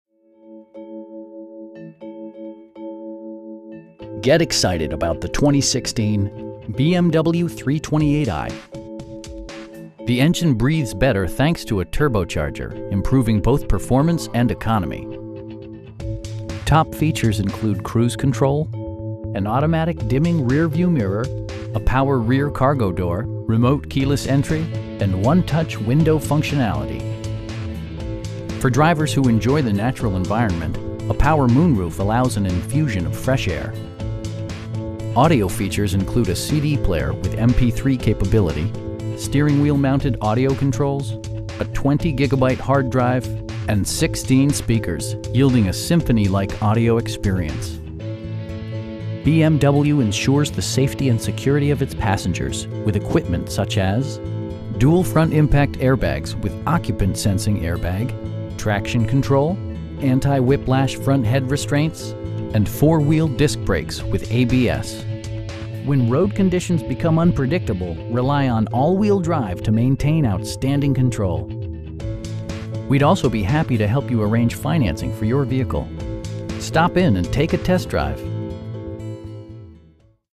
2016 BMW 3 Series Gran Turismo 328i xDrive
10720 Northlake Auto Plaza Blvd

Get excited about the 2016 BMW 328i. The engine breathes better thanks to a turbocharger, improving both performance and economy. Top features include cruise control, variably intermittent wipers, adjustable headrests in all seating positions, an automatic dimming rear-view mirror, heated seats, a power rear cargo door, remote keyless entry, and 1-touch window functionality. With high intensity discharge headlights illuminating your path, you'll always appreciate maximum visibility. For drivers who enjoy the natural environment, a power moon roof allows an infusion of fresh air. Audio features include a CD player with MP3 capability, steering wheel mounted audio controls, A 20 gigabyte hard drive, and 16 speakers, providing excellent sound throughout the cabin. BMW ensures the safety and security of its passengers with equipment such as: dual front impact airbags with occupant sensing airbag, head curtain airbags, traction control, brake assist, anti-whiplash front head restraints, a security system, an emergency communication system, and 4 wheel disc brakes with ABS. When road conditions become unpredictable, rely on all wheel drive to maintain outstanding control. We'd also be happy to help you arrange financing for your vehicle. Stop in and take a test drive!